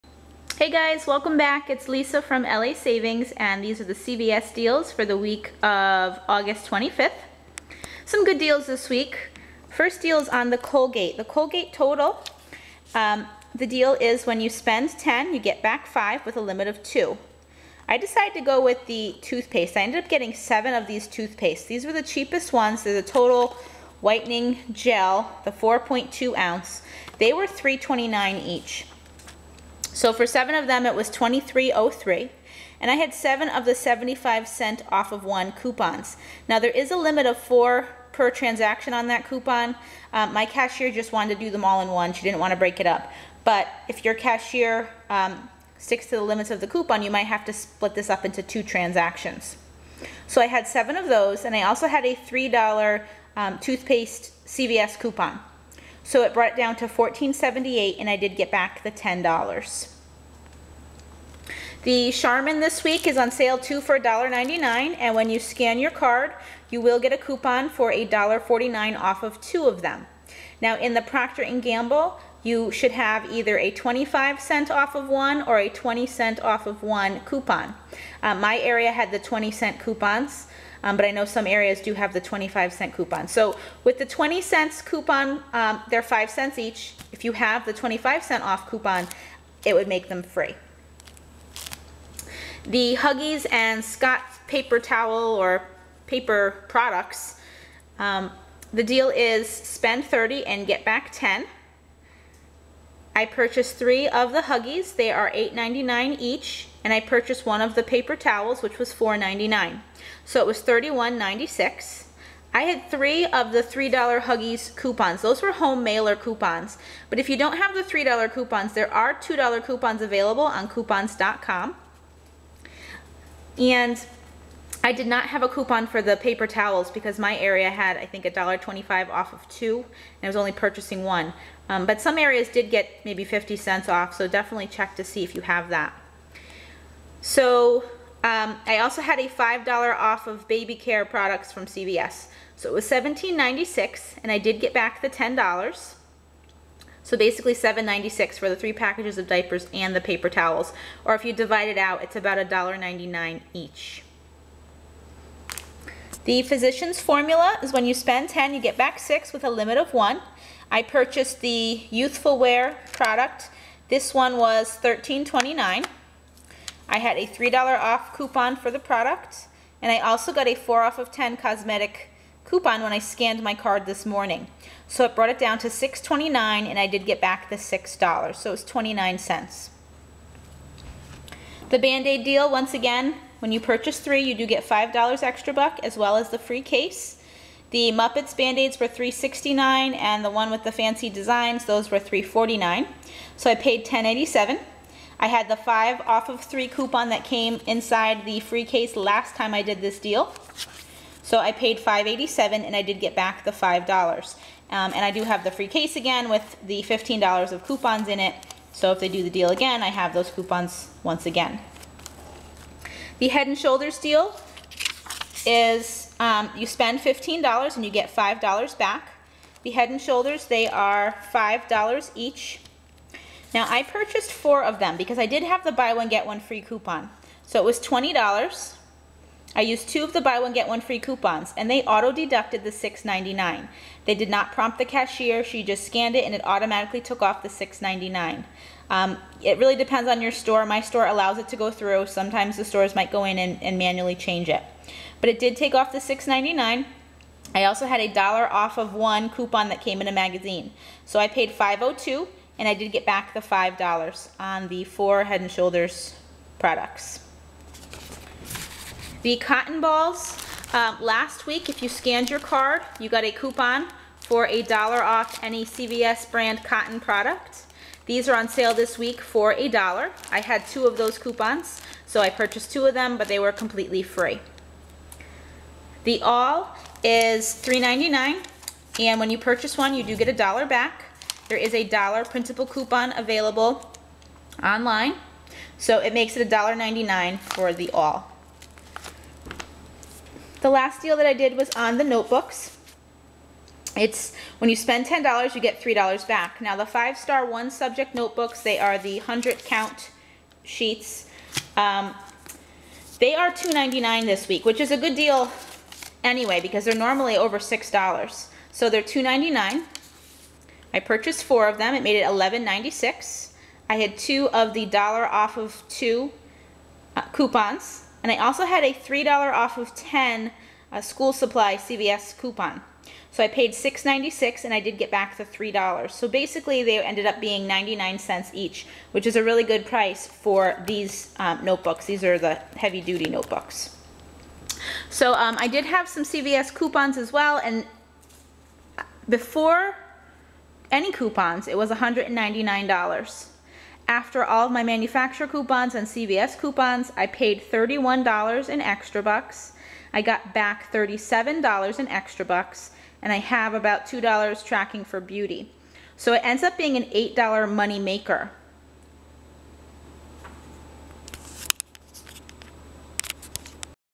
CVS Deals 8 25 2013 - CVS Haul Cheap Diapers, Shampoo & More!!
These are the CVS deals for the week of August 25th.  Some good deals to be found this week.  Enjoy!

Sign up for the ibotta app to start earning extra money on the items you purchase.

Currently there is a deal for Schick razors which can be paired up with the sales this week!